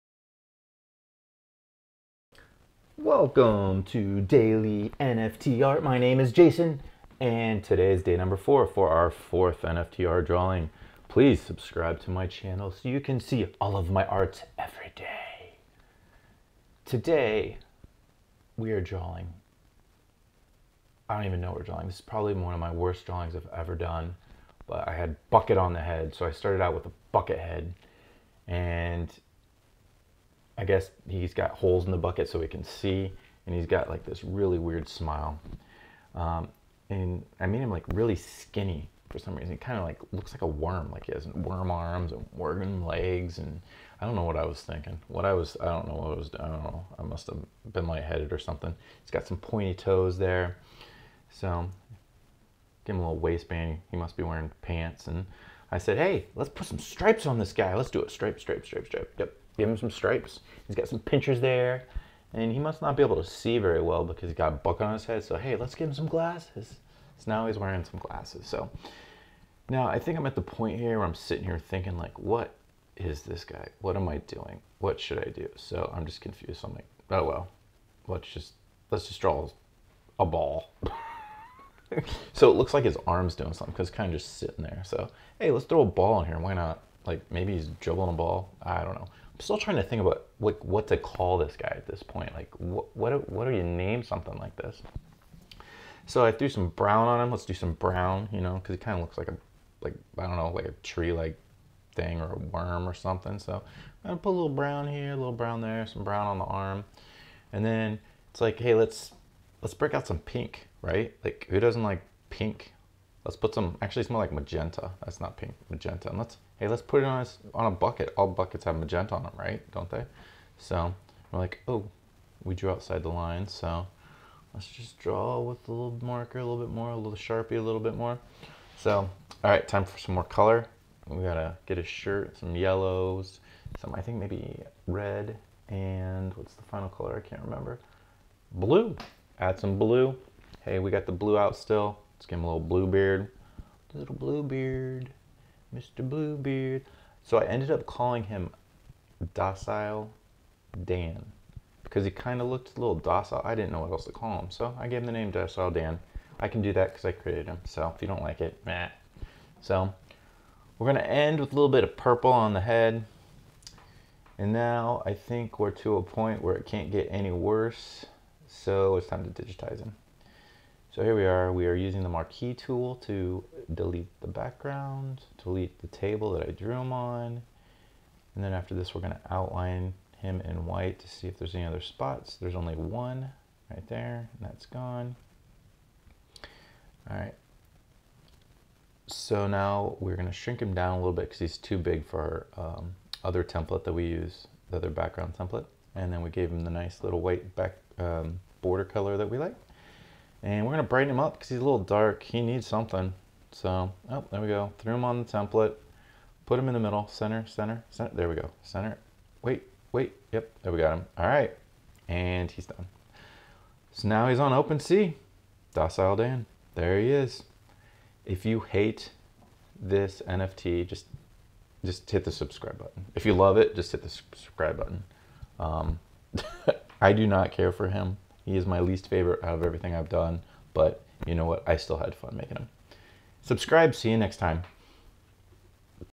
The Worst NFT Drawing Ever Made
This is my worst NFT drawing ever made for the channel. Using a black sharpie I draw a cartoon creature name Docile Dan and turn him into an NFT on the Ethereum blockchain using Opensea.io.

As you will see anyone with paper and markers can draw a simple, digitalize, edit and create an NFT.